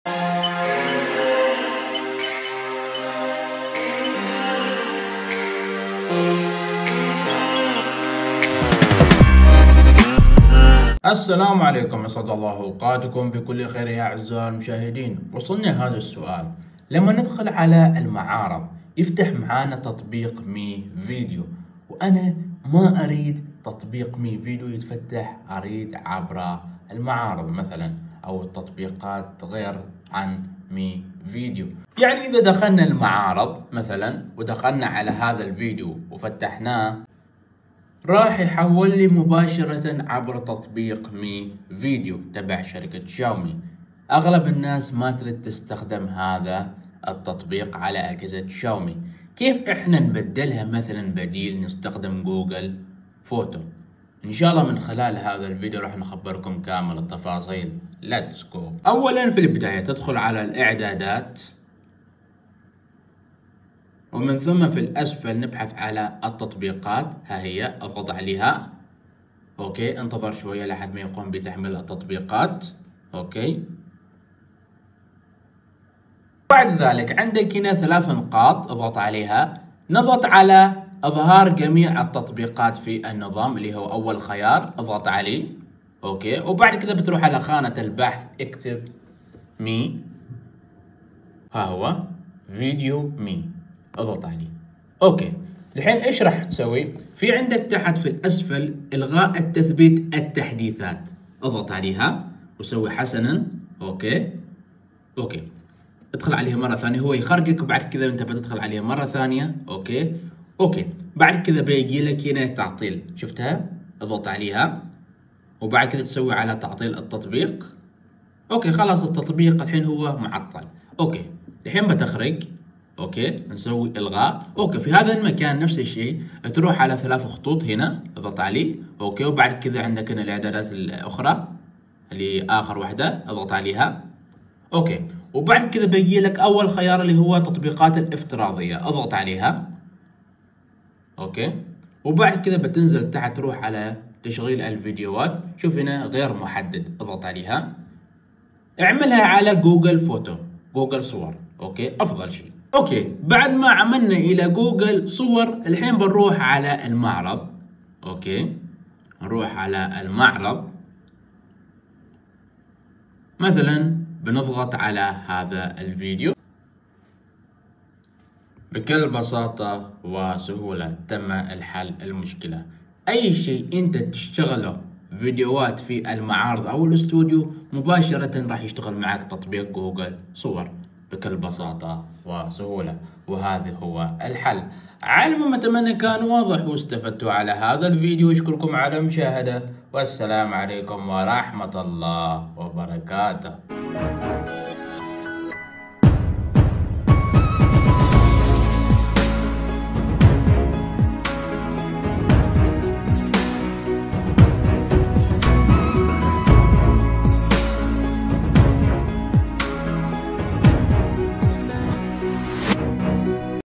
حل مشكلة تطبيق فيديو MI يفتح بشكل تلقائي عندما تقوم إعادة تشغيل الفيديو على استوديو على هاتف شاومي
Twitter ( تويتر )

Instagram ( الانستجرام )

WhatsApp ( واتس اب )

Telegram ( تيليجرام )

Kwai ( كواي )

الاختصار الفيديو

‏المقدمة : 00:00

البدء : 00:59

النهاية : 03:05